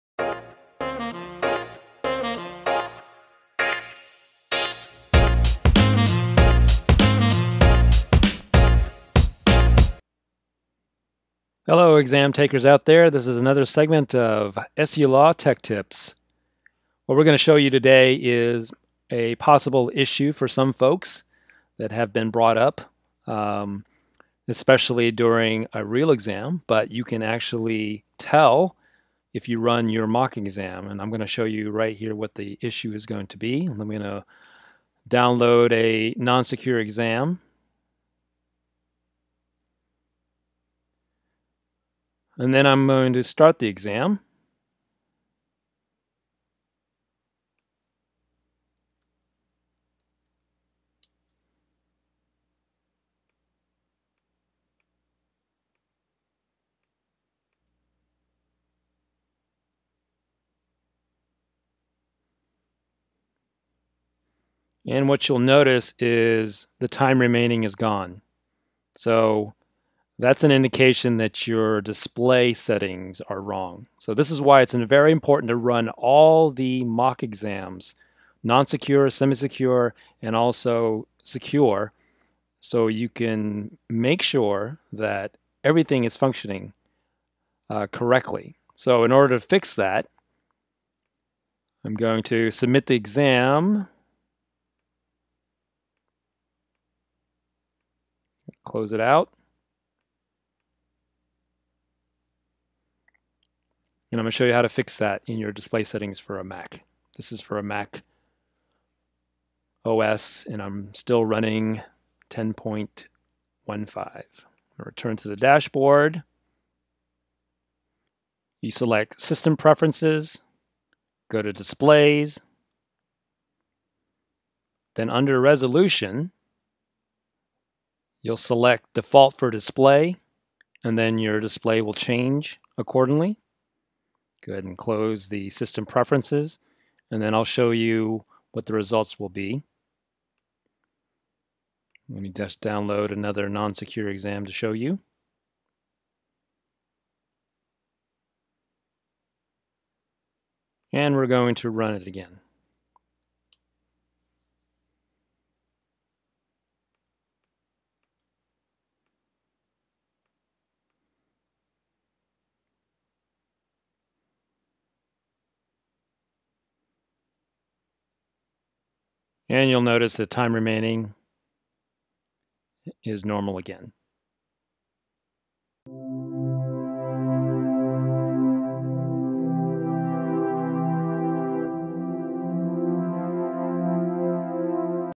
How to check and fix the missing "Time Remaining" issue in Examplify
If you have noticed the Time Remaining is missing from the top of your Examplify screen, then it is your display settings. We'll show you how to check by running mock exams and then fixing the issue on your Mac OS display settings.